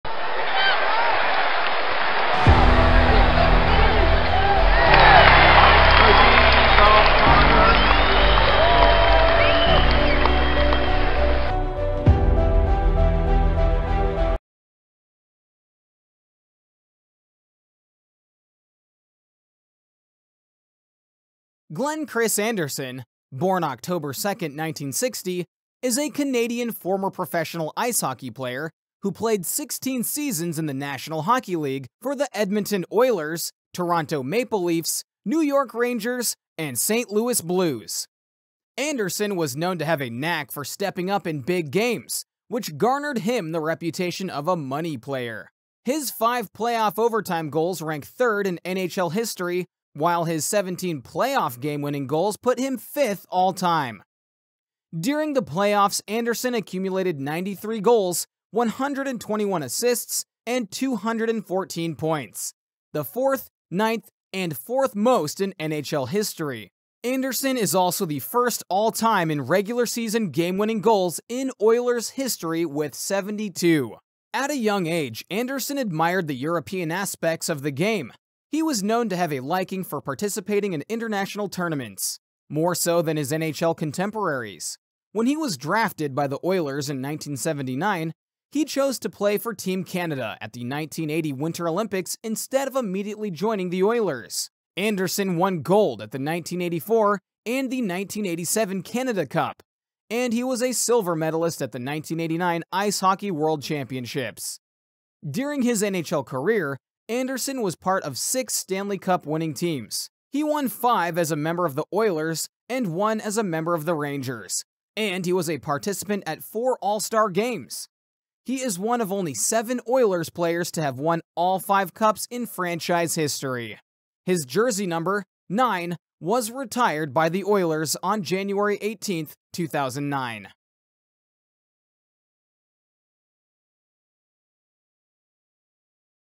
GLENN ANDERSON NHL HALL OF FAME CAREER HIGHLIGHTS
Glenn Chris Anderson is a Canadian former professional ice hockey player who played 16 seasons in the National Hockey League for the Edmonton Oilers, Toronto Maple Leafs, New York Rangers, and St. Louis Blues.